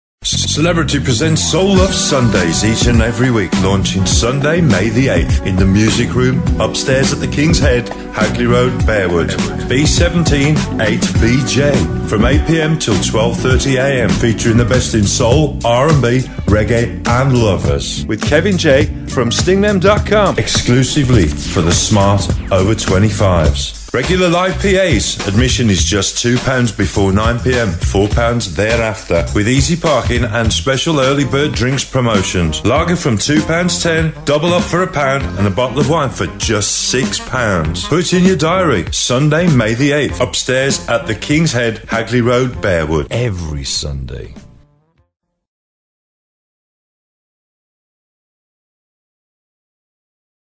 Soul Luv Sundays Kingshead.wmv
Soulebrity Presents Soul Luv Sundays
Launching 8th May 2011 8pm - 12.30am
£2 before 9pm £4 after.
Upstairs in the  music room
Kingshead Hagley Rd Bearwood B17 8BJ